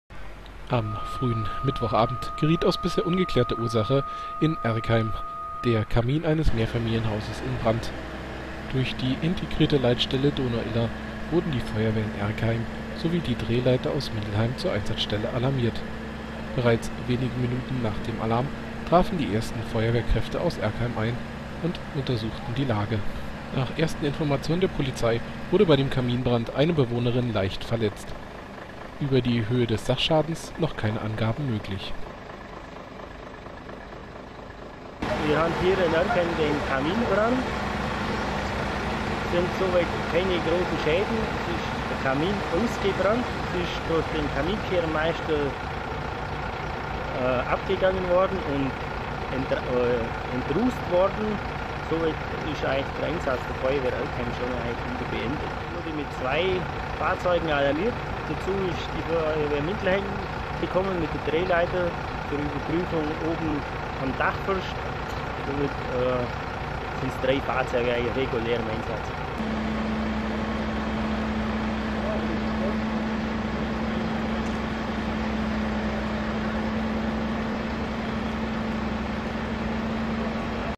Kaminbrand Erkheim
Kurz nach 17:00 Uhr alarmierte ein Erkheimer Hausbesitzer über den Notruf 112 die Feuerwehr, nachdem er bemerkt hatte, dass sein Kamin brennt. Nur wenige Minuten später heuelten in Erkheim die Sirenen der Feuerwehr, zeitgleich piepsten die Funkmelder in Mindelheim.